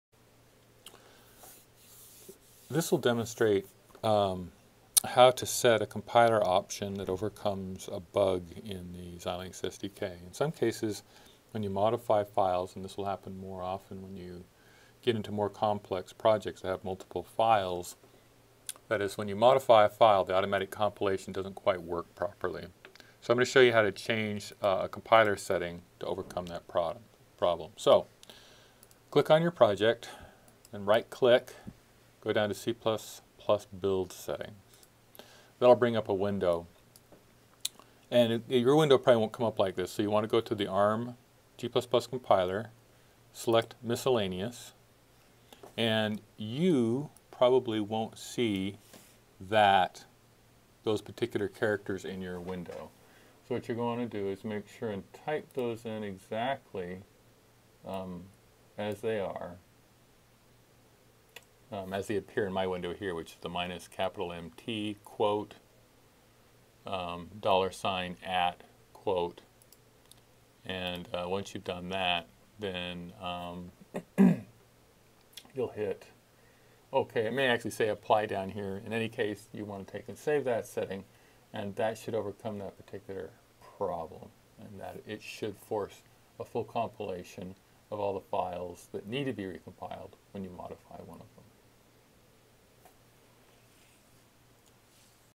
BYU ECEN 330 - V13 Modify Compiler Setting to Overcome Xilinx SDK Bug
Demonstrates how to modify a compiler setting to overcome a bug in the Xilinx SDK.